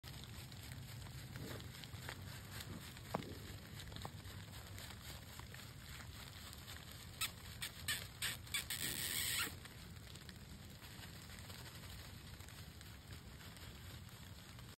Red Fox in the Snow While White-Tailed Deer Hunting.
During an all day sit in the saddle I not endured the first snow of the season but got to see this cool male red fox.